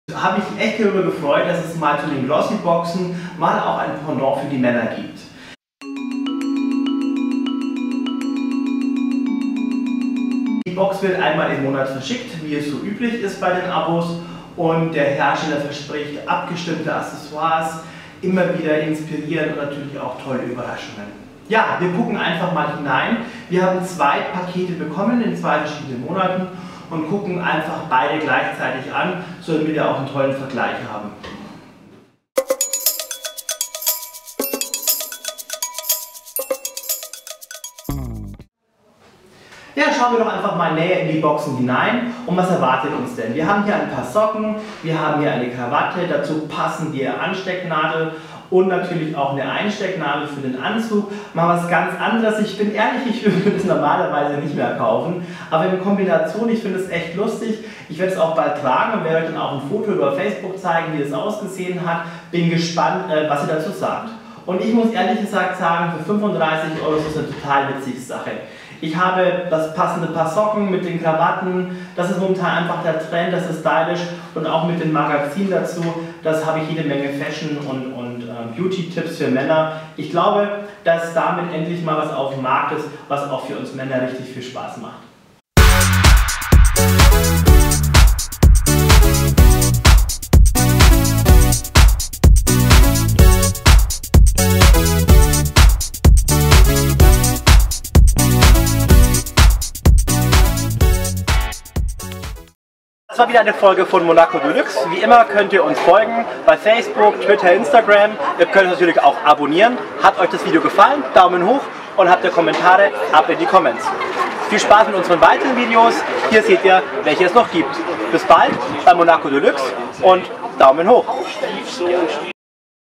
GentsBox, Testvideo, Fashion-Box im Abo
Die GentsBox ist eine exklusive Fashion Box nur für Männer! Krawatten, Fliegen, Einstecktücher, Lapel Flowers, Armbänder, Krawattenklemmen, Burlington Socken und auch immer ein variierendes Beautyprodukt – also alles, was das Männerherz begehrt.